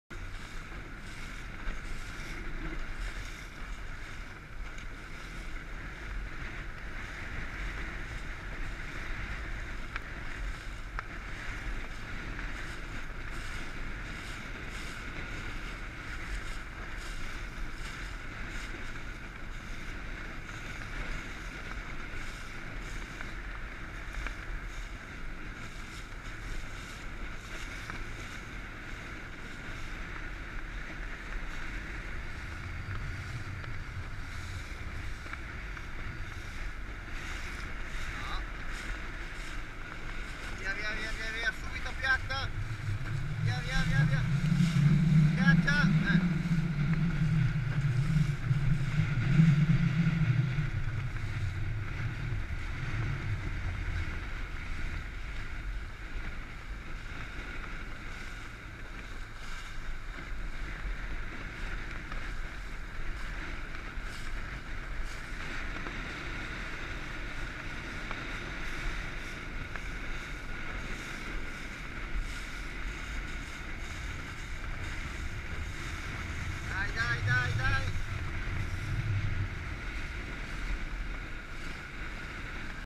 Margaret - Allenamento Voltri
Allenamento Laser su campo Voltri domenica 24 maggio 2015